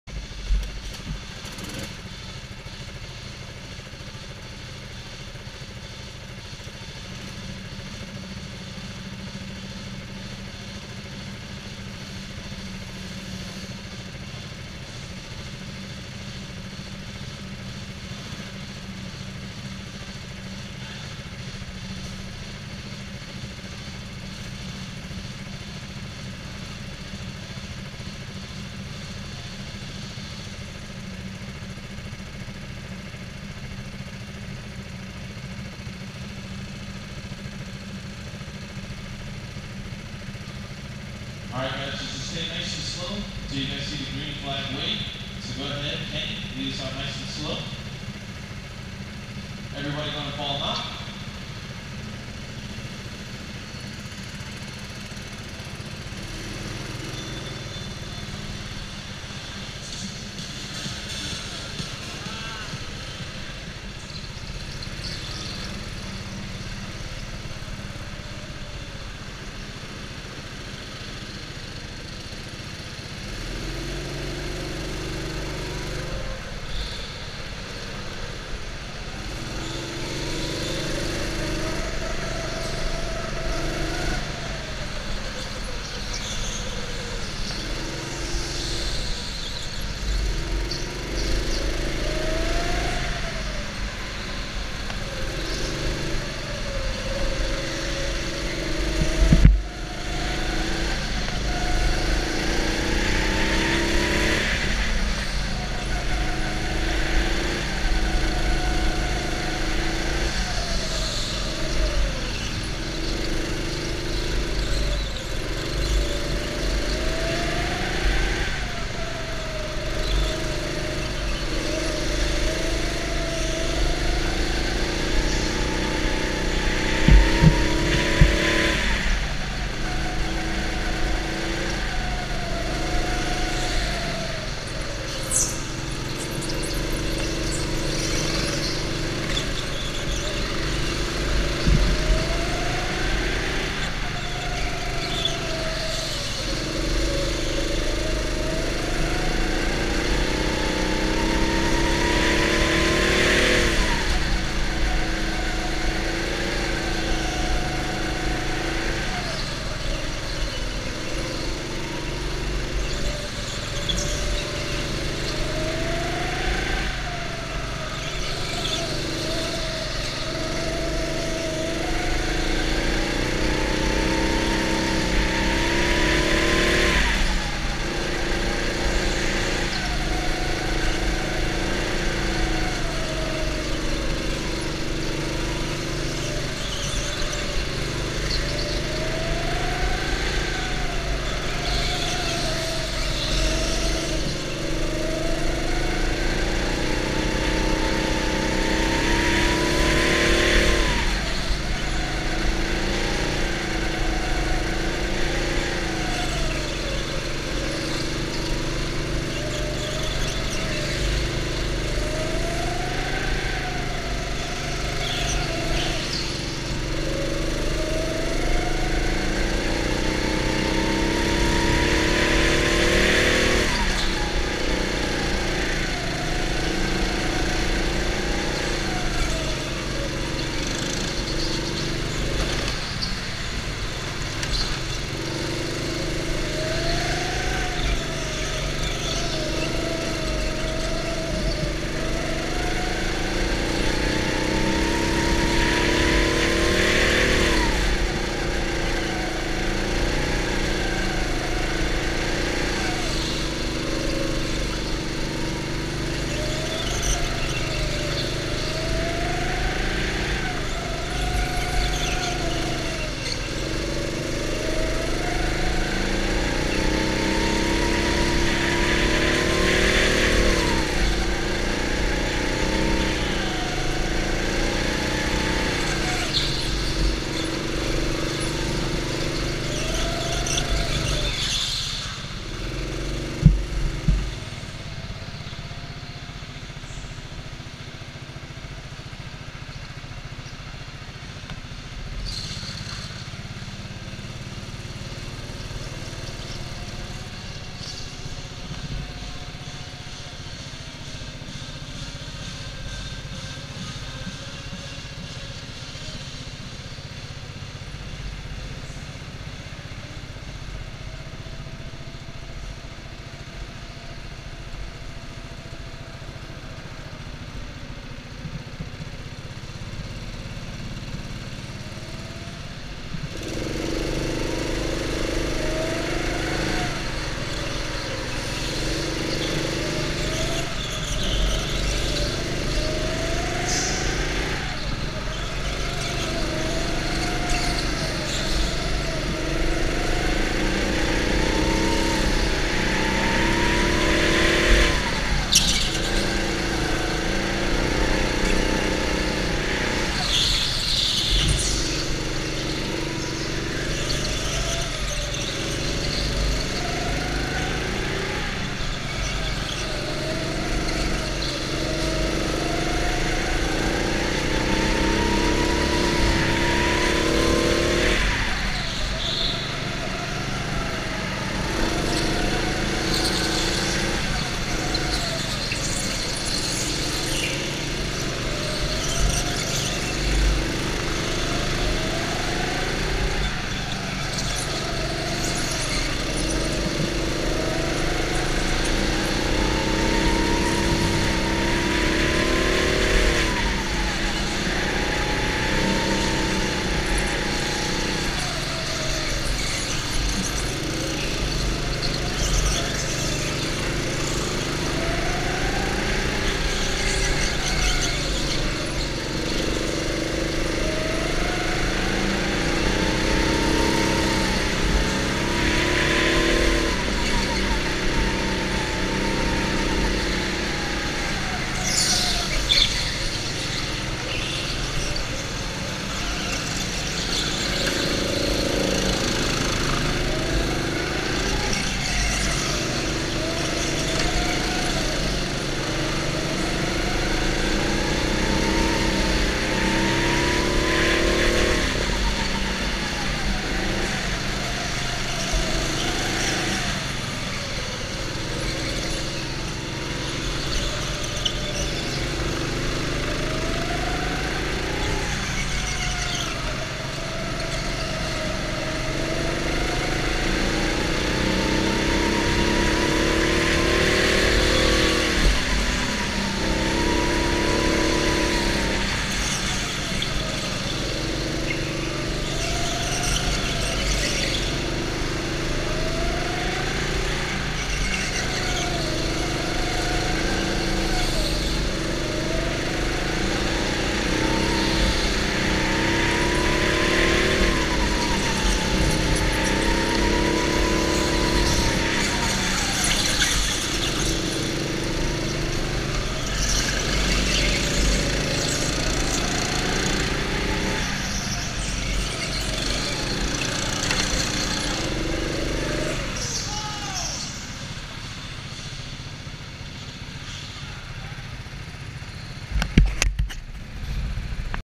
LeMan Karting, Fremont (CA) August 2nd 2014 - GoPro Hero3+ Testing
New GoPro camera - time for action stuff to do! So let's go Go Karting then! Awesome afternoon with looots of fun and some cool videos and pics. Camera was fixed on helmet top.